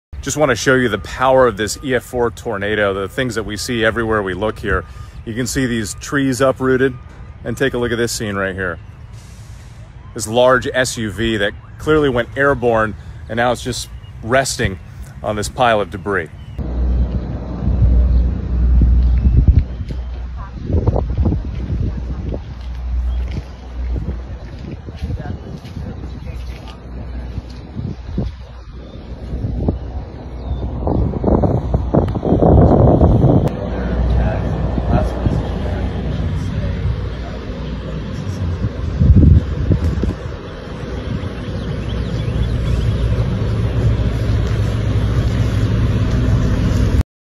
Whit Johnson shows tornado damage in Rolling Fork, Mississippi
As ABC News Journalist Whit Johnson was in Rolling Fork, Mississippi to show these aftermath of tornado that has been damaged. mississippistrong mississippistrong❤️

Thanks to the Instagram Stories: @whitjohnsontv